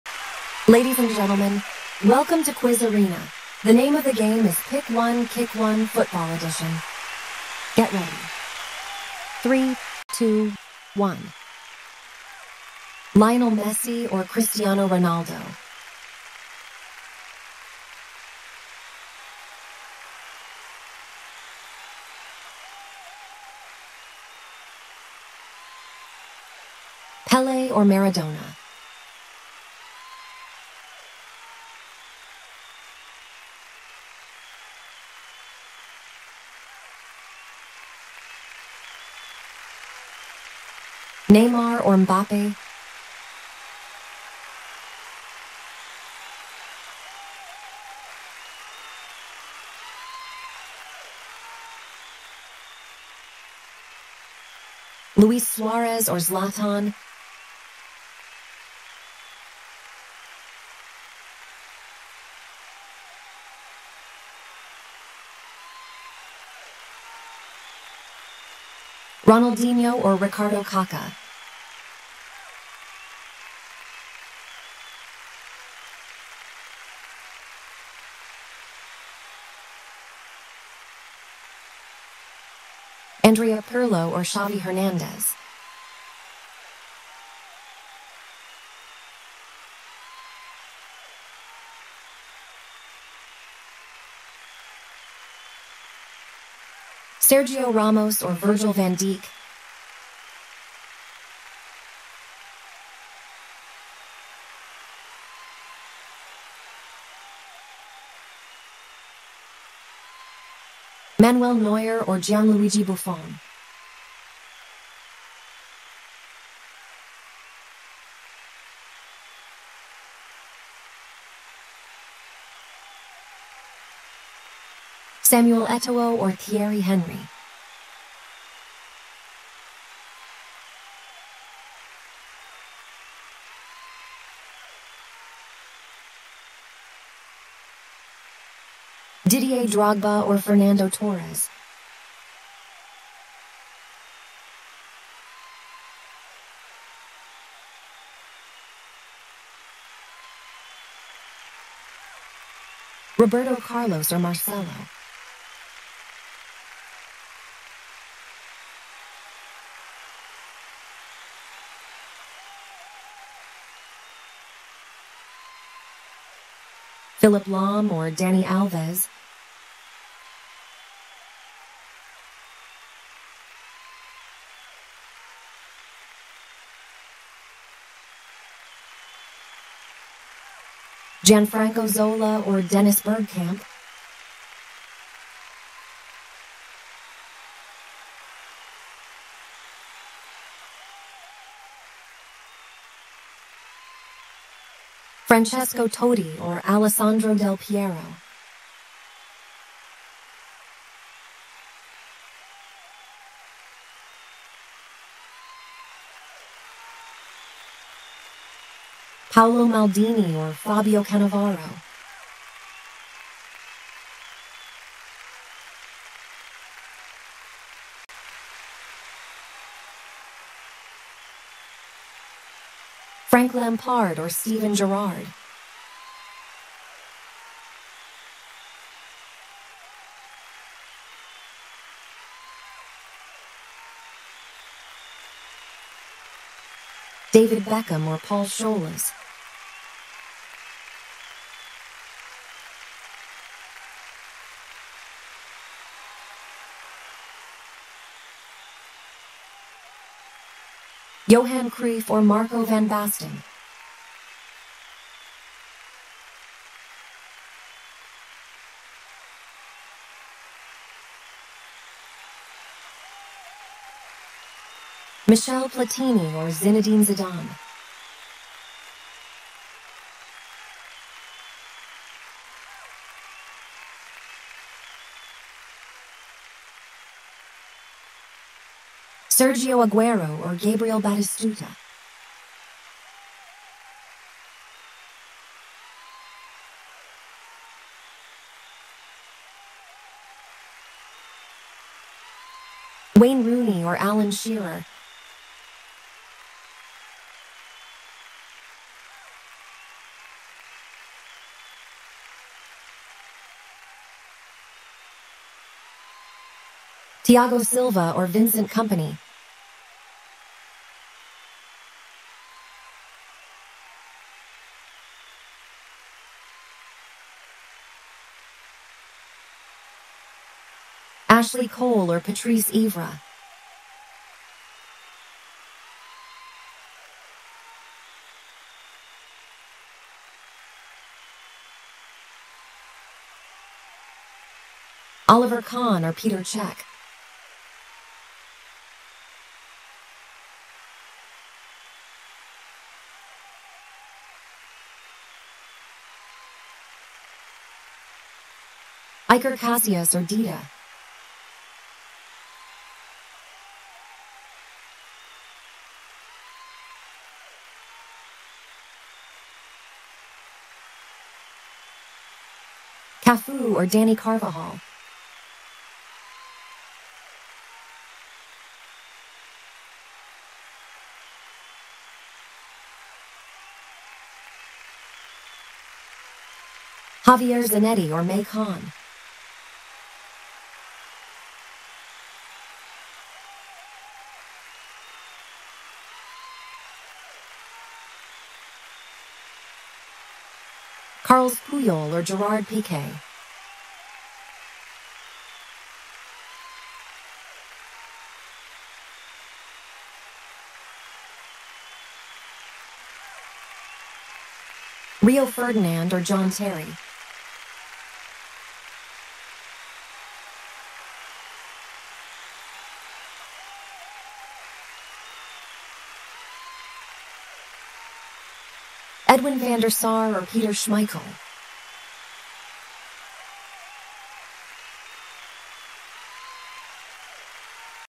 Pick One Kick One | ULTIMATE FOOTBALLER Edition
"Get ready to kick off the ultimate showdown on the virtual turf with 'Pick One Kick One: FOOTBALLER Edition'! 🏆⚽ Who's got the slickest skills and the wildest goals? Dive into this electrifying game where you'll have to make the toughest choices – pick your favorite football legends and decide who kicks their way to victory! ⚡🥅

From Messi's mesmerizing dribbles to Ronaldo's gravity-defying headers, we've got all the A-listers lined up for you. But beware, it's not just about the pitch prowess; it's about style, charisma, and pure swag! Will Pele outshine Neymar in this fierce face-off? Will Maradona show Beckham who's boss? The decisions are gut-wrenching, and the excitement is off the charts!

But hold up, it's not all serious business – prepare for some epic banter and a good laugh along the way. Our quirky commentary will keep you in splits as you agonize over each selection. And hey, we're not just hitting you with a game; we're gifting you a masterclass in football history and pop culture!

Did we mention that this video is an SEO jackpot? And hey, if you find yourself yelling at the screen, 'No way, how could you not pick Zidane?!' – don't worry, you're not alone!

Get your snacks ready, call up your mates, and let the 'Pick One Kick One: FOOTBALLER Edition' frenzy begin! Score big on fun, knowledge, and a whole lot of virtual adrenaline. ⚽🎮 Don't wait, join the action!" 🚀🎉